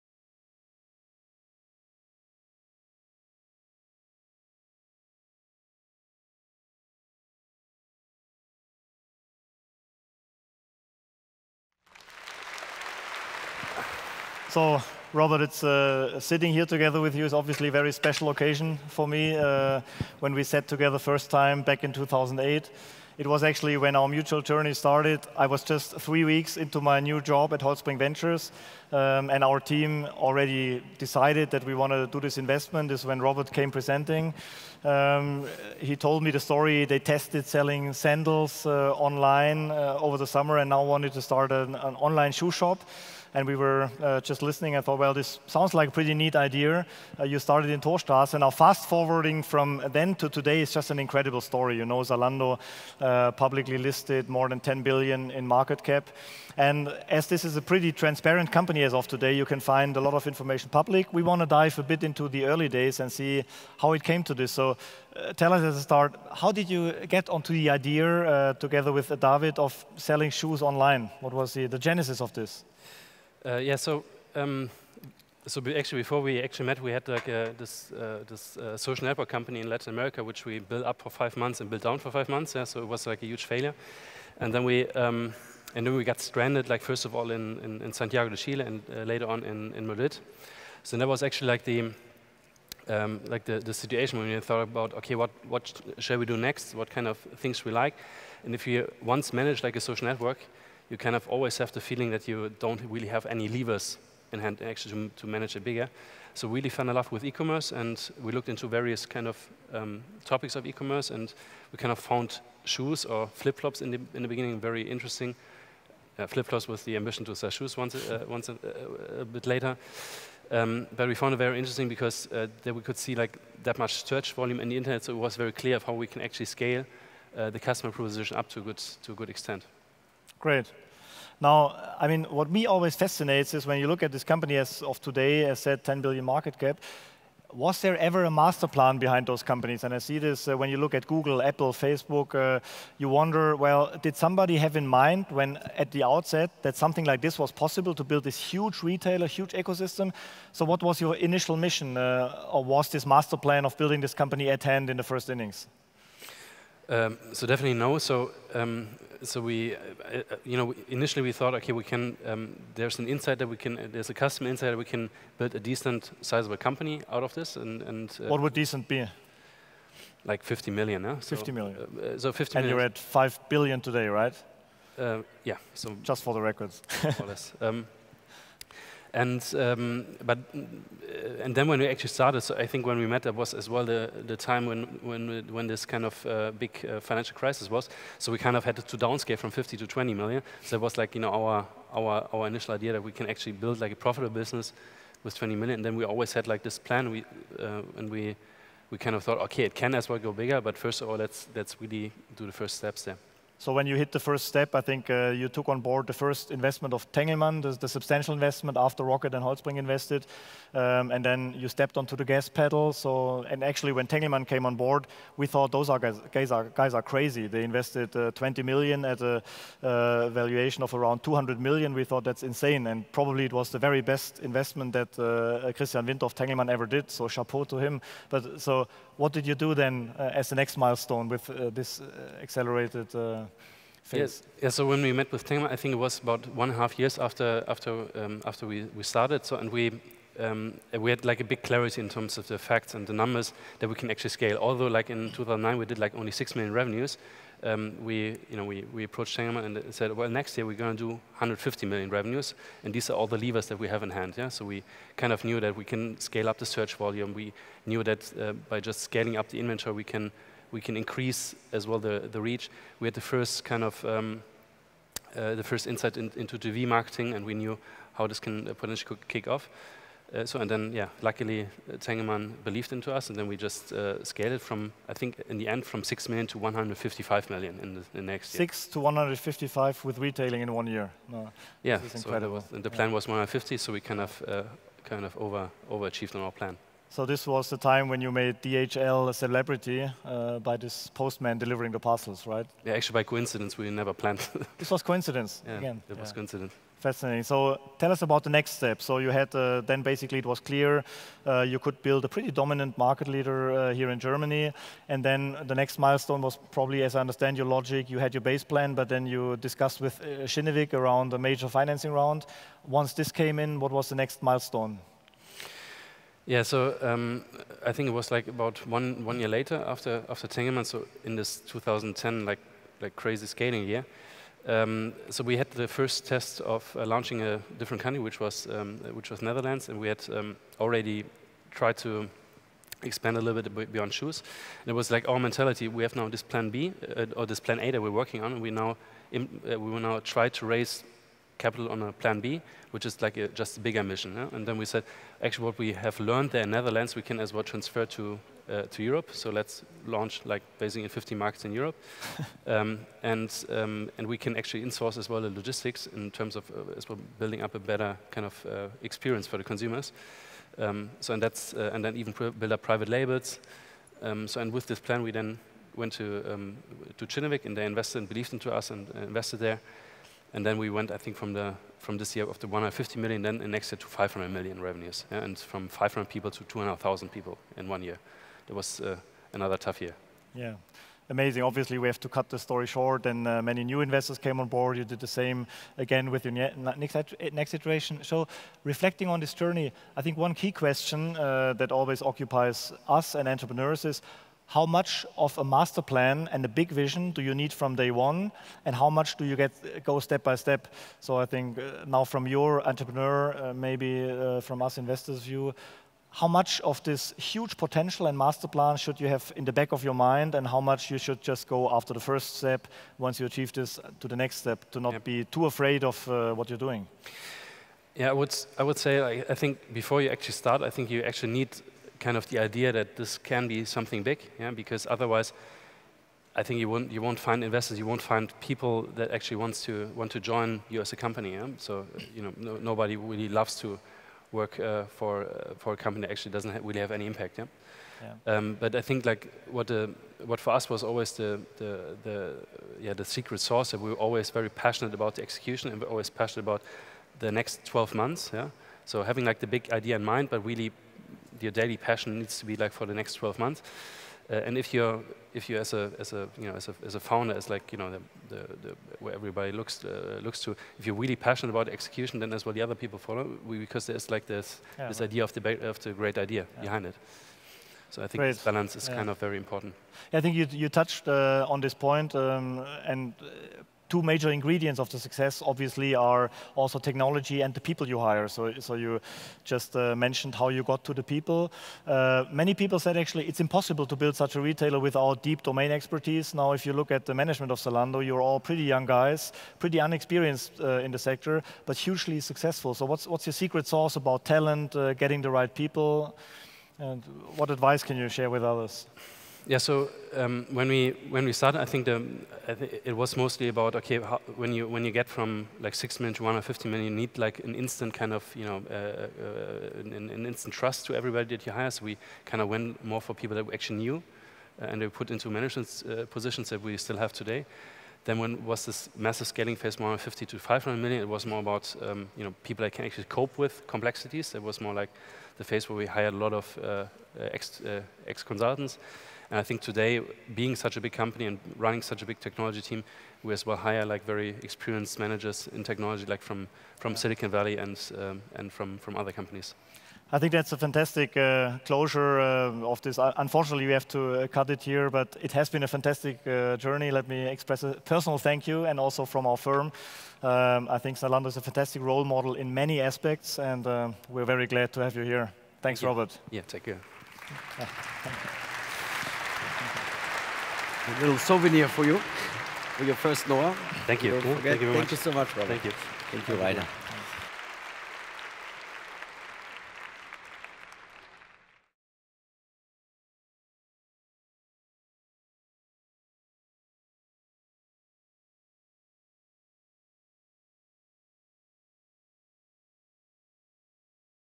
Fireside Chat: Zalando & HV Holtzbrinck Ventures - NOAH17 Berlin
Fireside Chat with Robert Gentz, CEO of Zalando and Rainer Maerkle, General Partner at HV Holtzbrinck Ventures at the Axel Springer NOAH  Conference 2017 in Berlin, Tempodrom 22-23 June 2017.